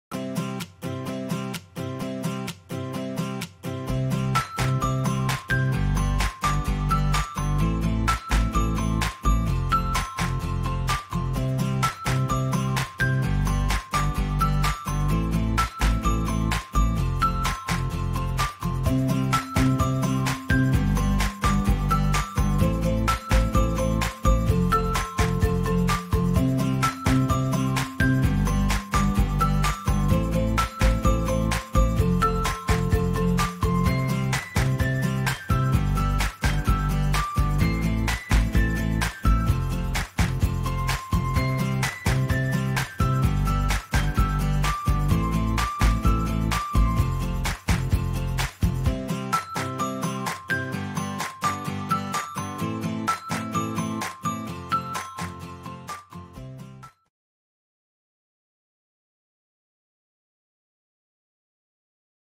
Tutorial panel interactivo empleo (Power BI).
Descubre el nuevo Power BI de Plataforma Economía de Jalisco COVID-19, en el encontrarás información relevante respecto a la situación actual del mercado laboral.